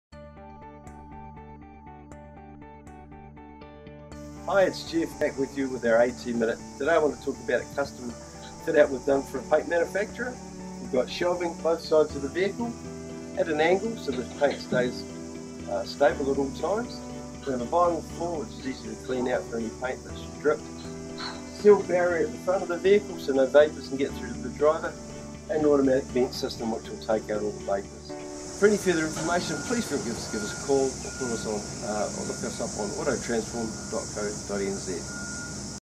AT Minute - Paint manufacturer fitout
This is Auto Transforms latest paint manufacturer fitout. This fitout allows for easy access and ensure the job is done at a fast pace and is safe